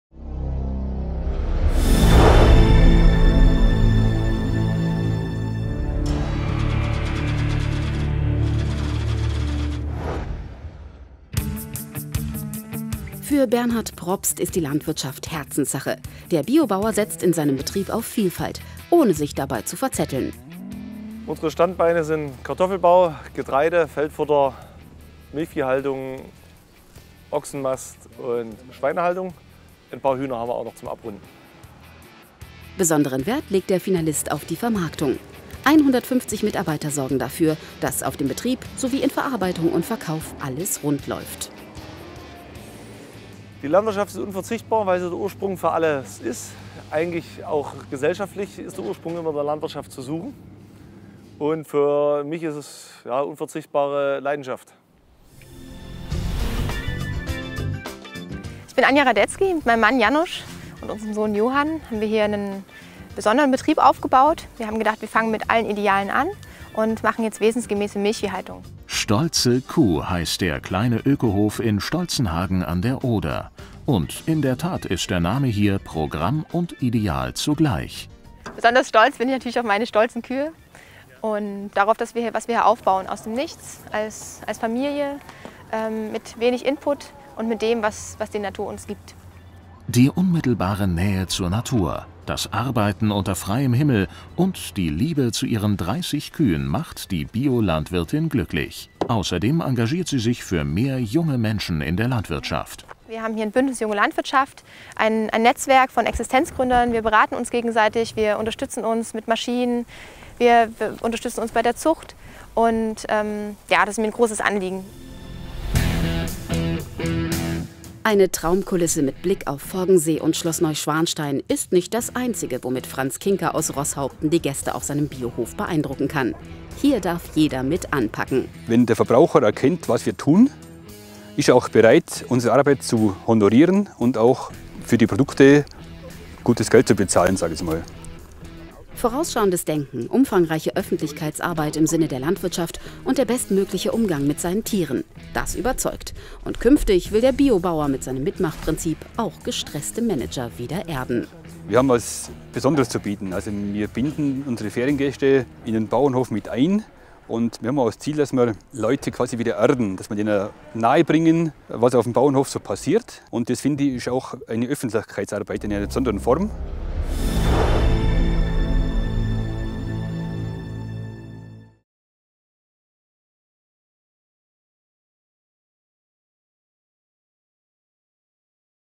CeresAward 2016 - die Kategorie Biolandwirt
Der Ceres Award, initiiert von der Redaktion des dlz agrarmagazin, wurde am 18. Oktober 2016 in Berlin zum dritten Mal verliehen. Der wohl bedeutendste Preis der deutschen Landwirtschaft zeichnet außergewöhnliche und erfolgreiche Landwirtinnen und Landwirte aus und wurde in diesem Jahr in 10 Kategorien vergeben.

Kandidaten in der Kategorie Biolandwirt waren Anja Hradetzky aus Stolzenhagen/Brandenburg, Franz Kinker aus Roßhaupten im Allgäu und Bernhard Probst aus Dresden/Sachsen, der zum Sieger gewählt wurde. Lernen Sie hier alle 3 Kandidaten und ihre Betriebe kennen!